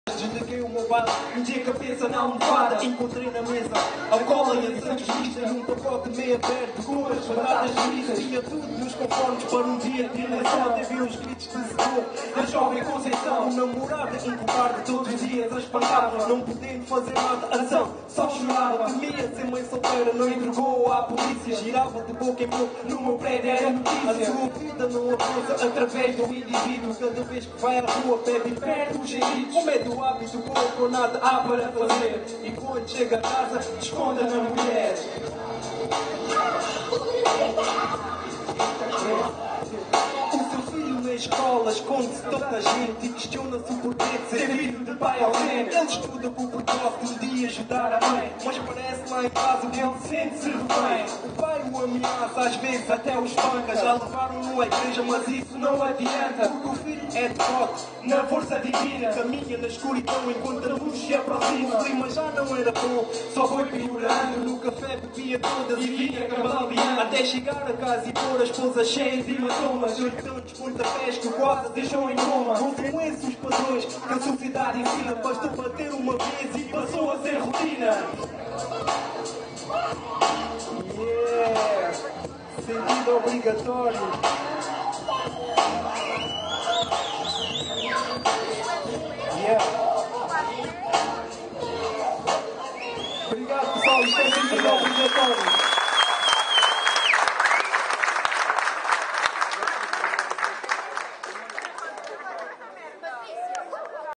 Concurso São Sebastião "Flow" - Actuação Neyd
Actuação do Neyd no concurso realizado no Atlantic Park Setúbal
Organizado pela Junta de Freguesia de S. Sebastião
Apoios:
Programa Escolhas Projecto VAI.PE - Escola Secundária D. João II; Atlantic Park Setúbal; Popular FM; Station loja de Setúbal; Setúbal TV; PageOne; Câmara Municipal de Setúbal.

O Objectivo deste concurso foi promover a sensibilização junto da população jovem para o tema a "Violência no namoro", através de um concurso onde revelámos novos talentos. As letras de rap na sua grande maioria são letras que descrevem a dura realidade das periferias; é um estilo musical conhecido como uma música de protesto apesar de nas suas origens também se falar sobre festas e diversão.
Neste caso o grande objectivo foi as letras falarem sobre o tema proposto. O que foi um grande sucesso os jovens trabalharam muito bem os seus trabalhos, tendo sido feito o espectáculo num lugar visível da nossa freguesia, para dar visibilidade a outro tipo de público, desmitificar que o rap não têm a ver com violência e Vandalismo.
Apesar de ter havido prémios para os três primeiros lugares, todos os 11 jovens participantes ganharam a oportunidade de serem entrevistados pela Popular FM para divulgação dos seus trabalhos e todos estarão em palco na Feira de Santiago em Setúbal , assim como a cobertura da Setúbal TV.